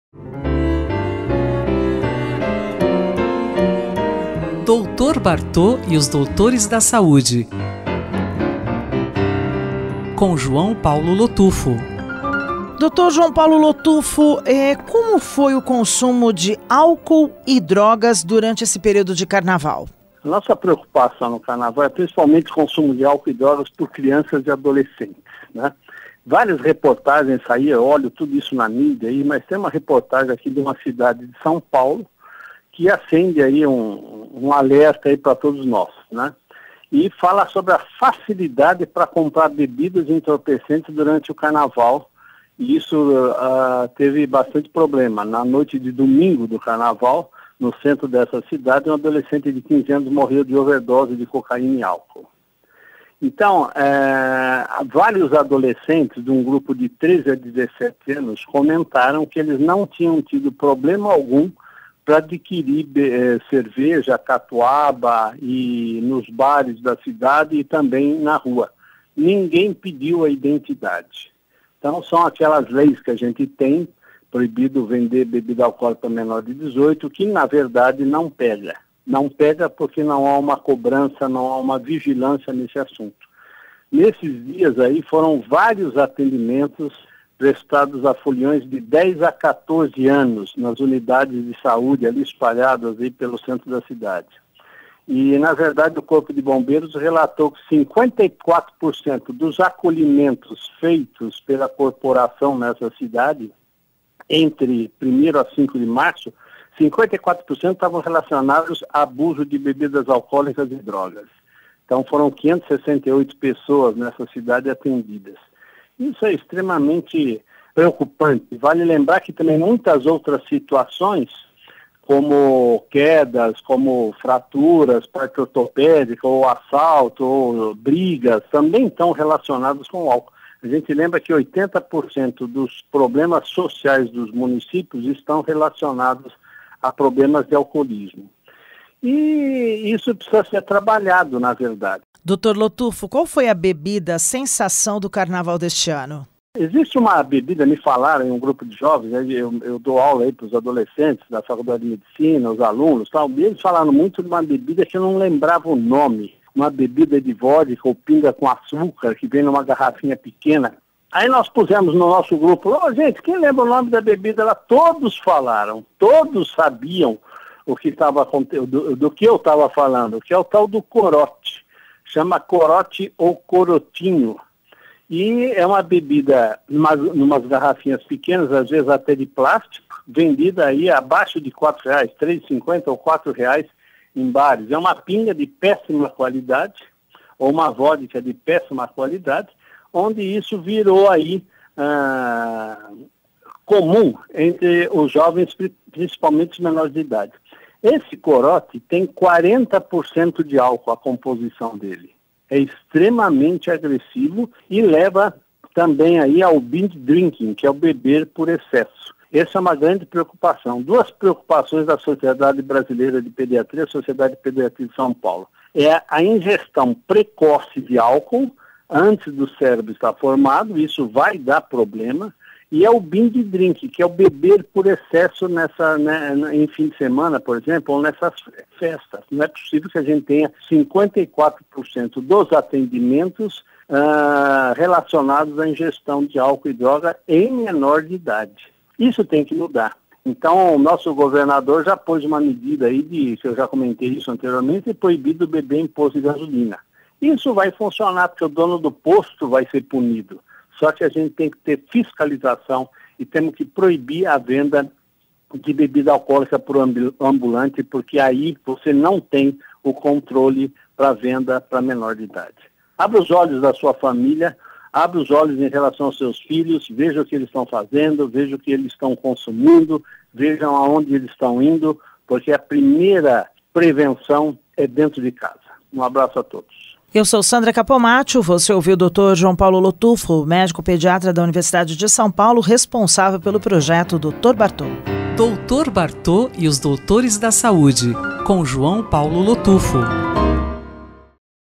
Carnaval e consumo de álcool e drogas por crianças e adolescentes [Dr. Bartô e os Doutores da Saúde]
Em Minas Gerais, um jovem de 15 anos morreu devido a uma overdose de cocaína

Playlist - [Dr. Bartô e os Doutores da Saúde] com João Paulo Lotufo

Baixe o aplicativo Jornal da USP, disponível para Android e iOS:
Google play